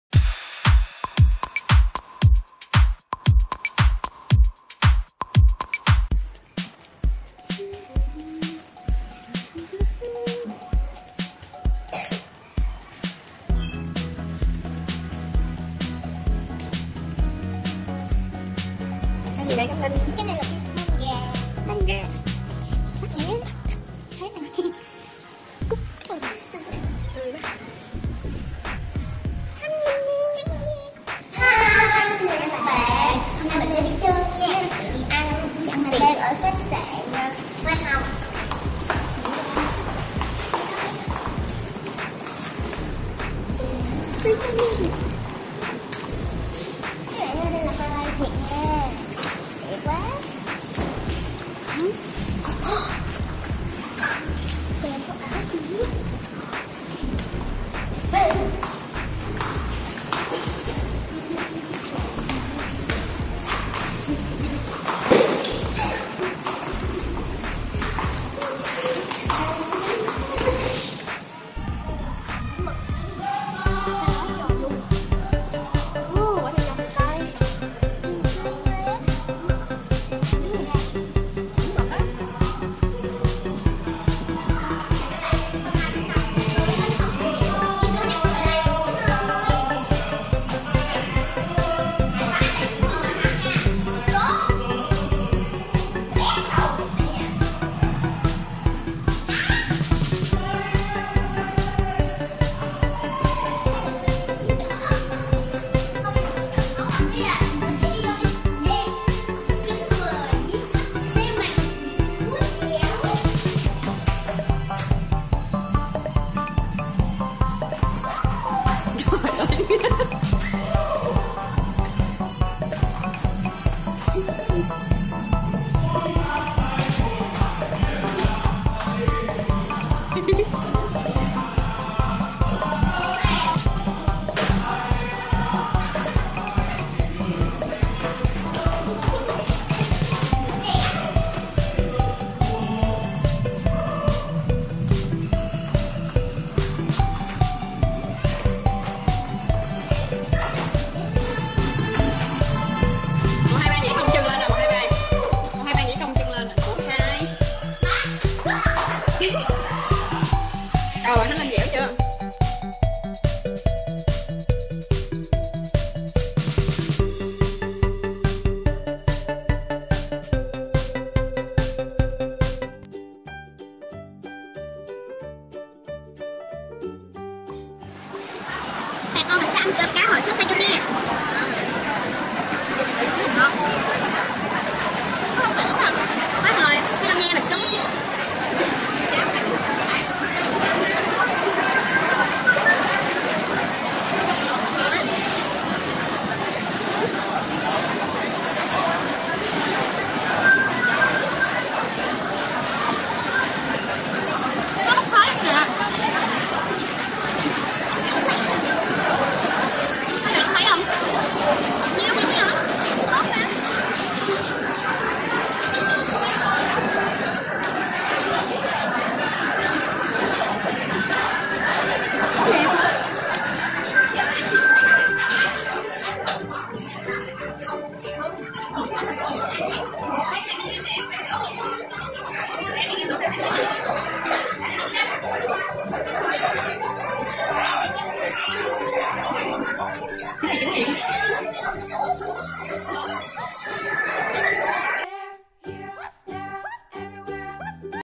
2019 | Vlog đi chơi cuối tuần của Bảo Minh, khám phá chỗ chơi mới là gì | Bao Minh Channel
2019 | Vlog đi chơi cuối tuần của Bảo Minh, khám phá chỗ chơi mới là gì | Bao Minh Channel

Hi, chào các anh chị & các bạn,
Hôm nay, mình tập thể dục chút cho vui nha.
Love you.